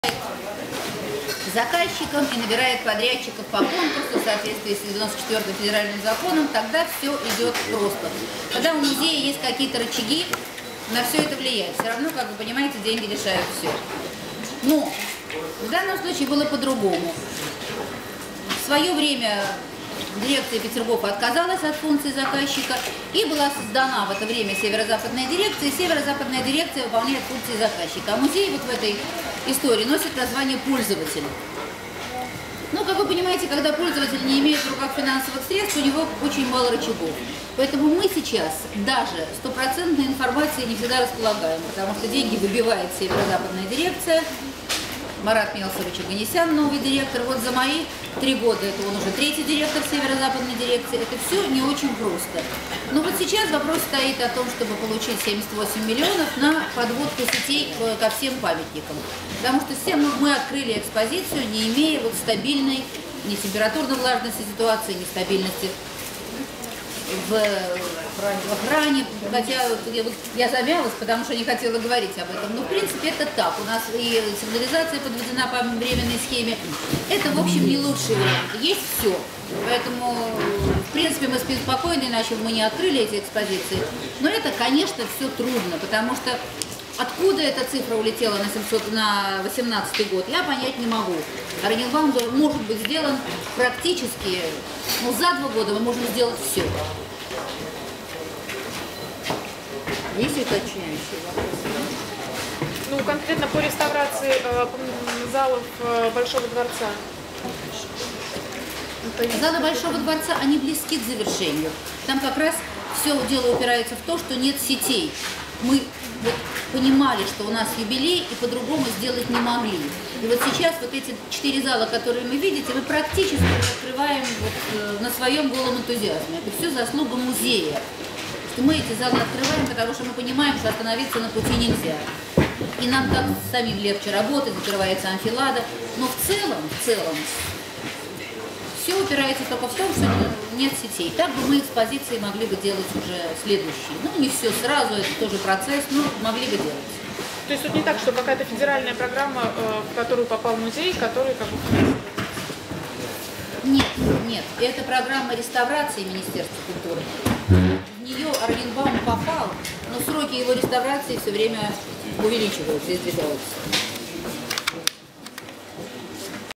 Директор ГМЗ „Петергоф" Кальницкая Елена Яковлевна
2 августа состоялось  открытие четырех залов второй очереди реставрации Большого Меншиковского дворца в Ораниенбауме. Этому событию предшествовал пресс-тур.